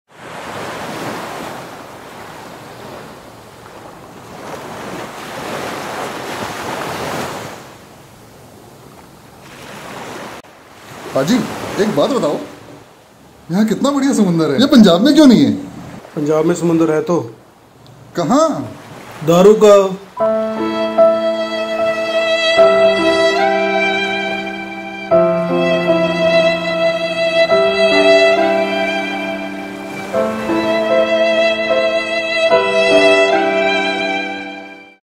Sea of Punjab ਪੰਜਾਬ ਦਾ ਸਮੁੰਦਰ पंजाब का समंदर | Funny clips hindi punjabi
Punjab also have a sea, even bigger than so called Big seas of the world, watch this clip to know what we are talking about

Channel name - Rabab Chandigarh

Credits -Inderpreet Rabab, Rohit Bhat, Akashdeep Gautam

Concept & Direction - Inderpreet Rabab

Label - Rabab Chandigarh